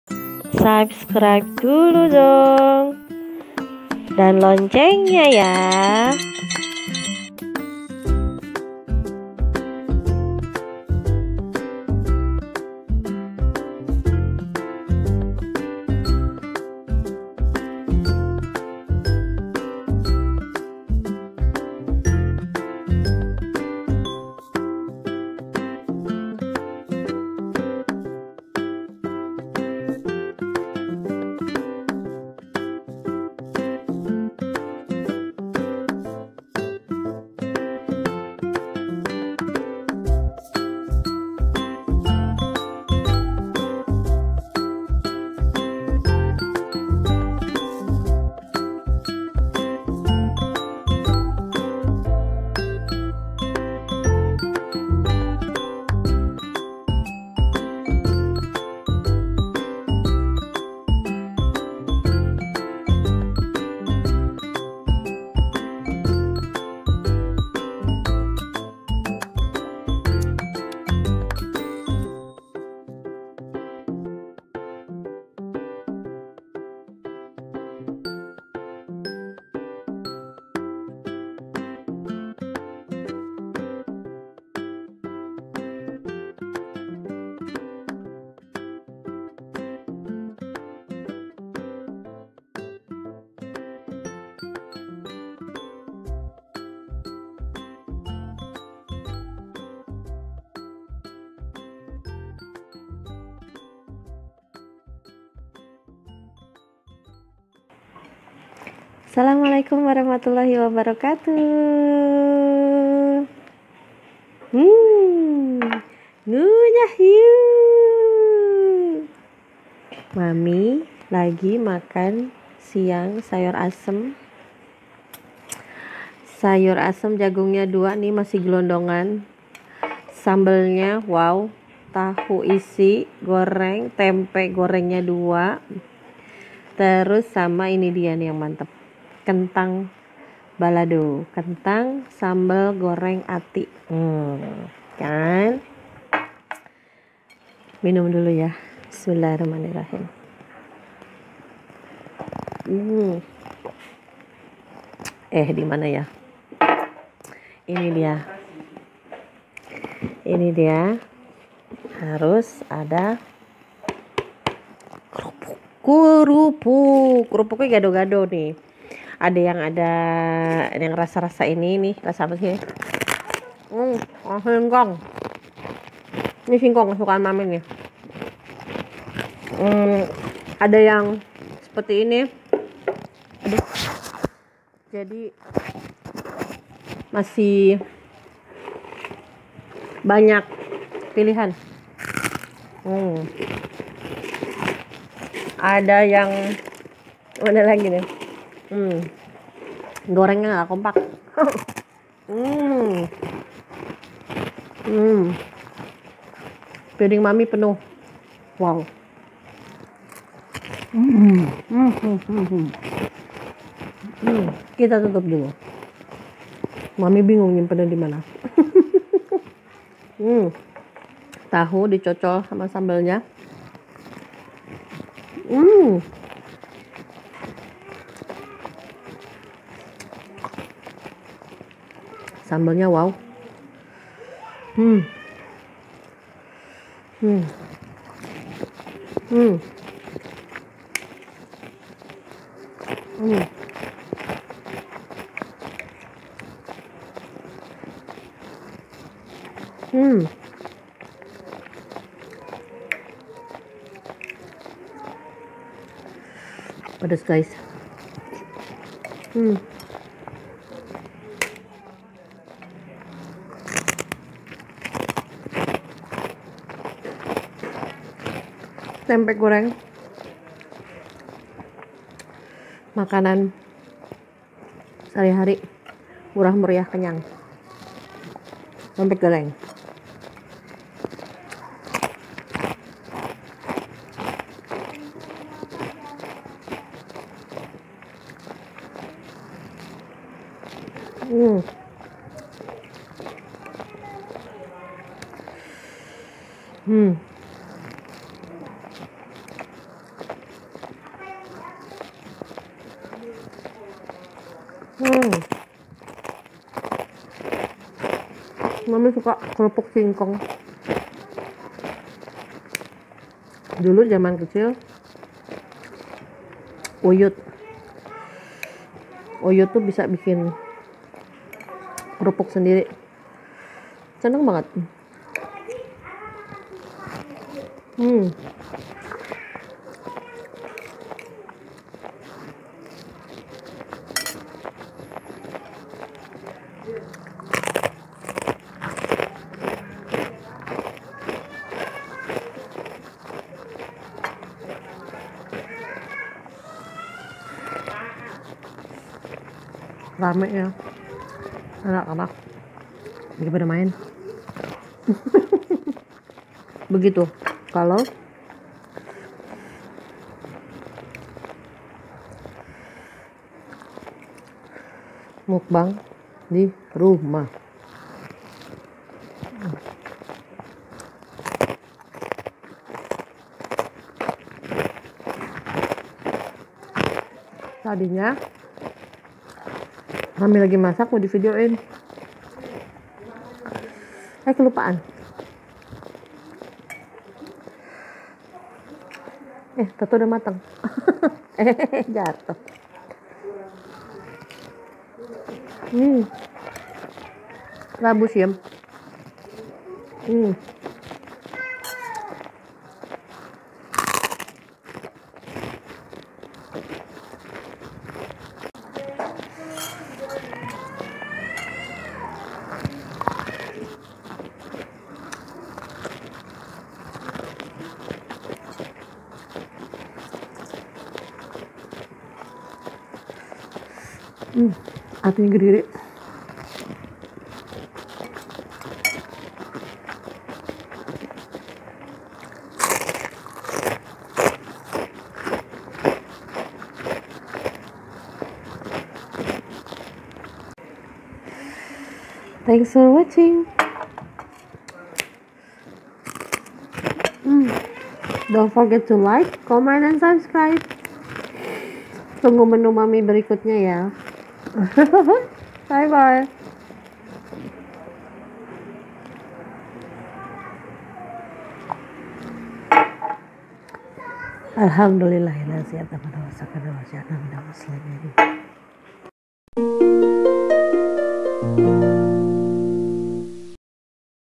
Makan tempe...murah.. meriah enak..
Alhamdulillah..momy yaya makan siang enak nih... nonton sampai habis ya...

support chanel momy yaya... dengan tekan lonceng nya...

momy yaya